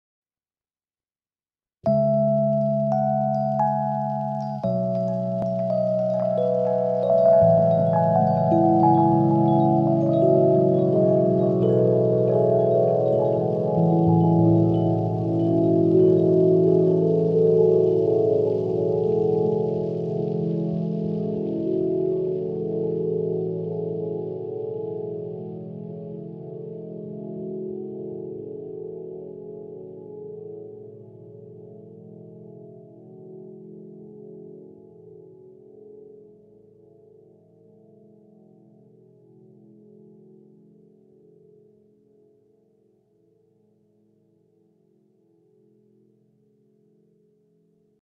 Tapographic Delay patch: Rupert's keys
A short recording of Rupert Lally on the electric piano, dissolved by the Tapo (factory tapography A2). Direct live recording, no overdub, no additional effects.